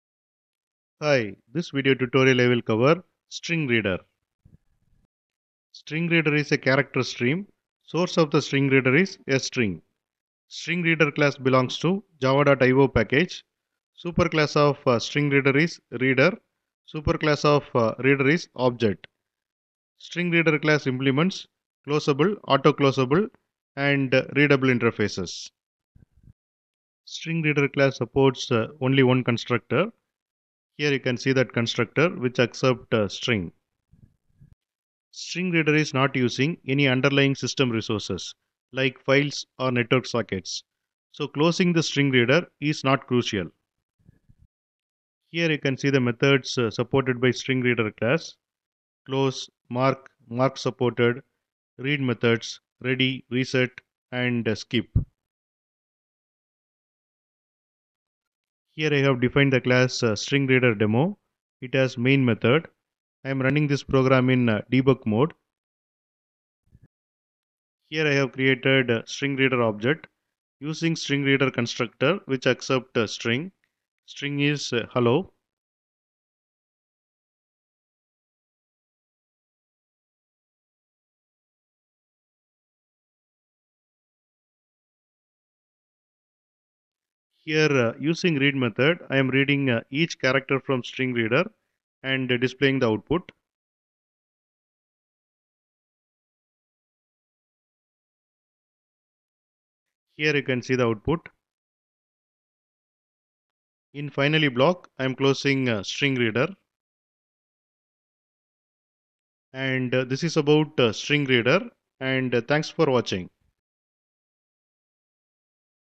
How to Use StringReader in Java | Java IO Tutorial
Welcome to this Java tutorial on StringReader! In this video, we dive into the StringReader class in Java IO. StringReader is a convenient way to read characters from a String as if it were an input stream.

### What You'll Learn:
- **Introduction to StringReader:** Understand what StringReader is and its role in Java IO.
- **How StringReader Works:** Explore how StringReader reads characters from a String.
- **Use Cases:** Learn about common scenarios where StringReader is useful.
- **Examples:** Hands-on examples demonstrating the usage of StringReader in Java programs.

### Why StringReader Matters:
StringReader is particularly useful when you need to treat a String as an input stream, allowing you to seamlessly integrate string-based data into your Java applications.

What is StringReader in Java? | Java IO | Java Tutorial